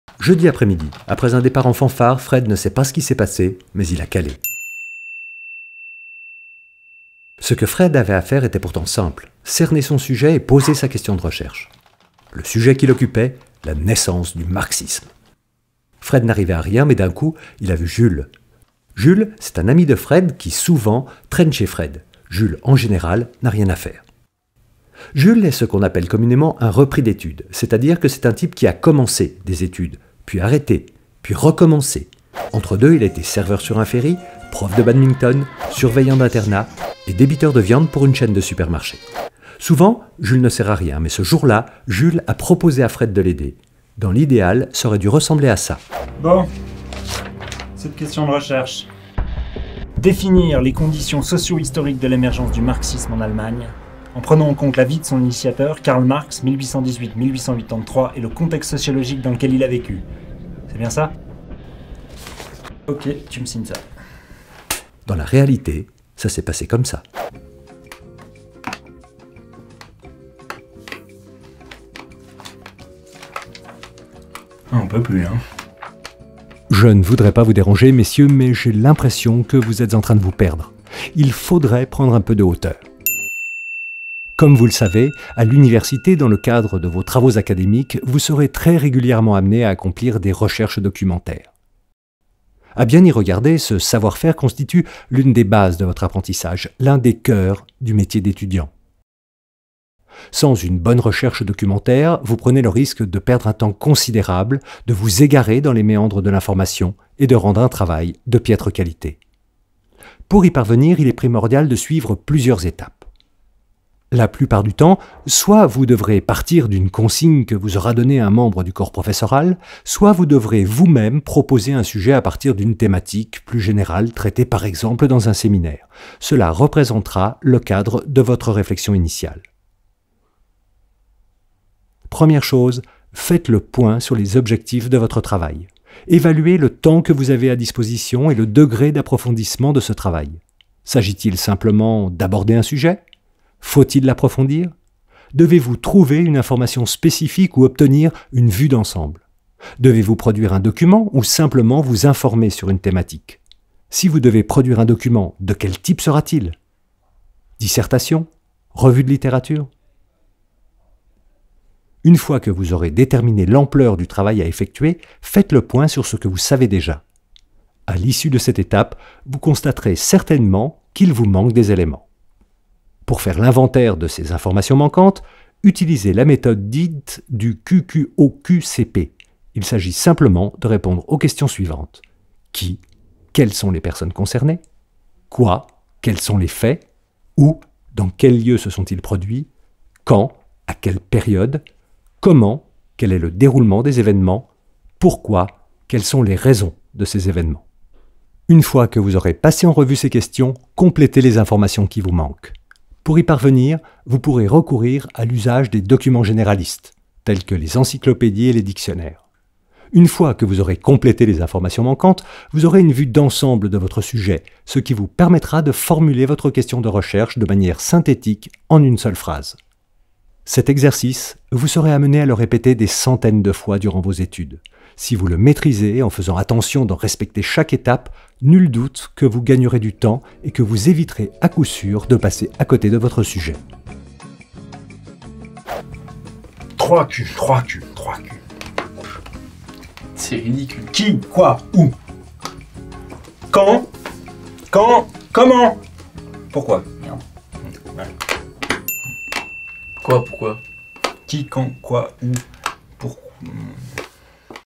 Les étapes essentielles pour aboutir à une question de recherche
Dans cette vidéo vous apprenez à cerner votre sujet pour un travail et à rédiger une question de recherche. Pour chaque leçon, du contenu additionnel (quiz, résumé textuel avec les éléments "à retenir" des leçons, et des ressources complémentaires, glossaire,...) est disponible

2016, Bibliothèque de l'Université de Genève